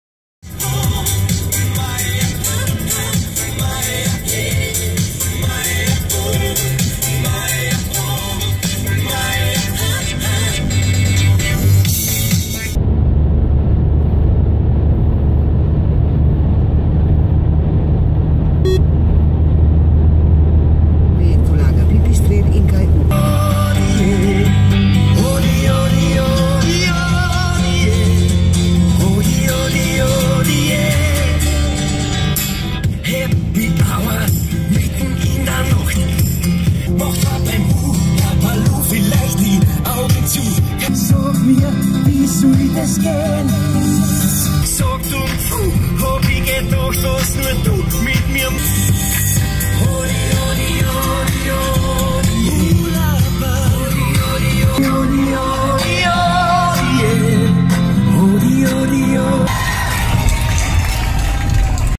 Hodiodiodiej we drive always with style FLIPP style Make different is your tradition
We presents Slovenia highway. We always drive with style.  Life needs memories! We can also make fotos and video for you. Make fotos is our tradition!